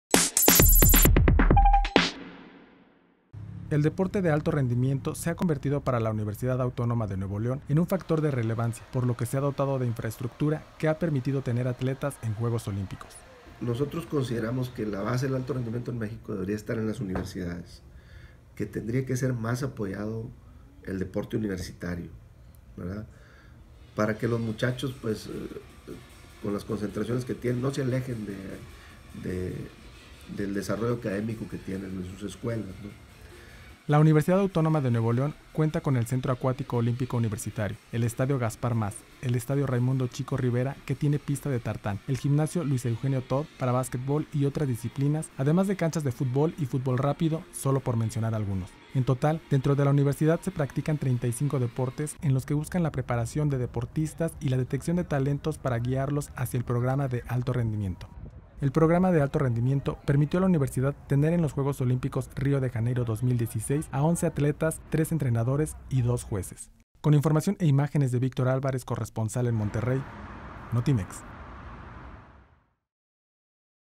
Busca la UANL impulsar a deportistas de alto rendimiento
El deporte universitario ha sido a lo largo de los años parte fundamental de la institución educativa, pero la mejora
de programas ha permitido que avancen en la preparación de los
atletas.